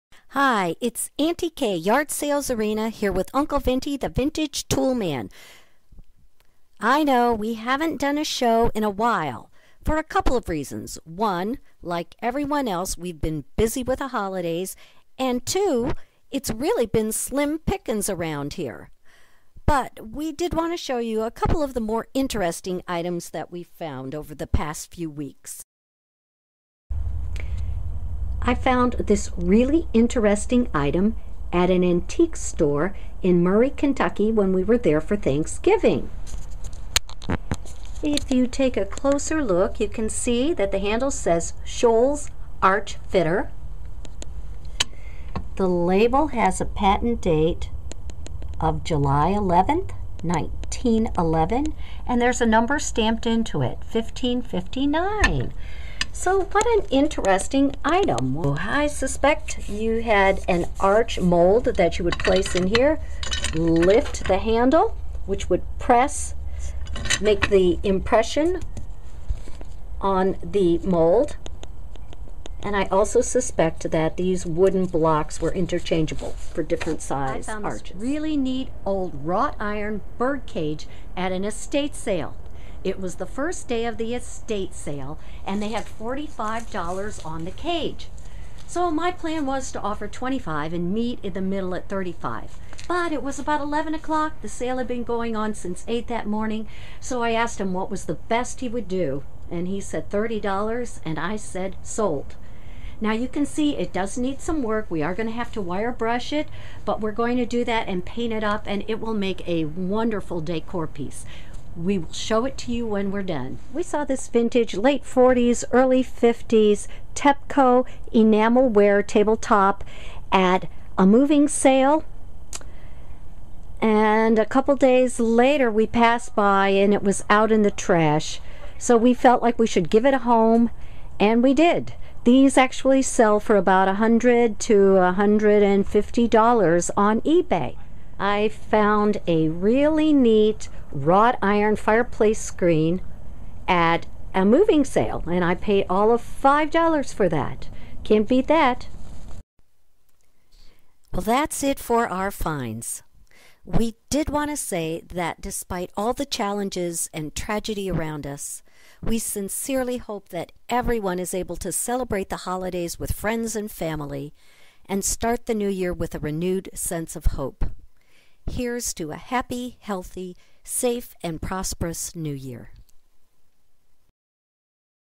THE YARD SALE SHOW: Christmas Eve, 2012
It's been slim pickens around here lately, but we wanted to share a few of our more interesting finds. Enjoy the Holidays. Here's to a happy, healthy, safe and properous new year!! Anti-que and Uncle Vintie